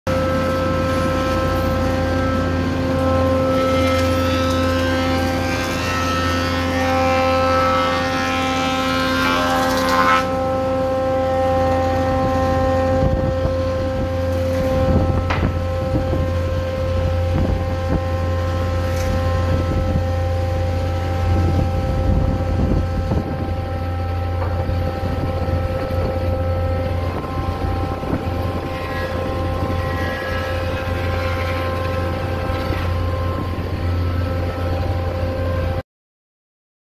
Fraser Woods planing wood
Fraser Woods can plane wood slabs from 2,2m wide to 5m long.
Wood planing of hardwood and sleepers is our specialty. Fraser Woods is a second-generation wood factory in Pretoria, Gauteng